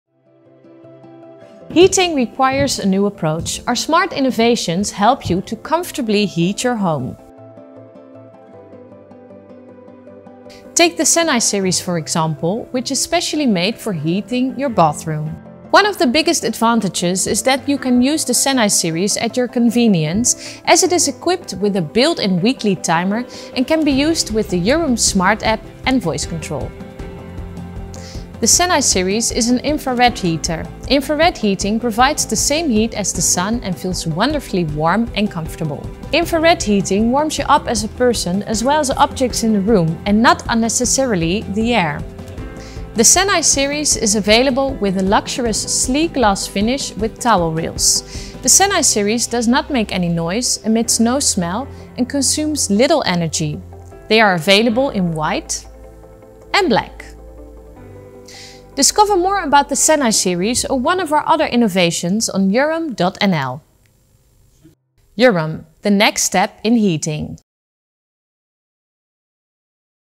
How does the Sani infrared bathroom heater work? (short - subtitles)
The Sani bathroom heaters work on infrared, and give the same heat as the sun. Perfect for the bathroom!